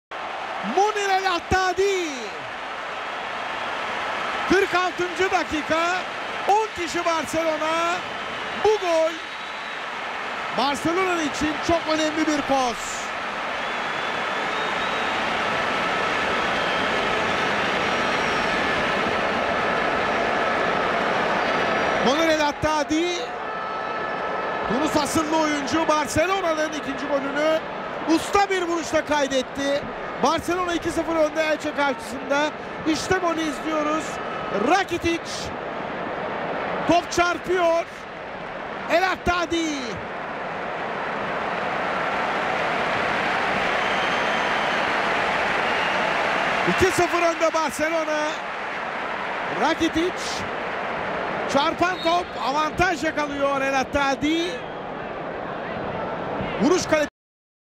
Munir El Haddadi debut FC Barcelona goal
Munir scored on his La Liga debut for FC Barcelona against Elche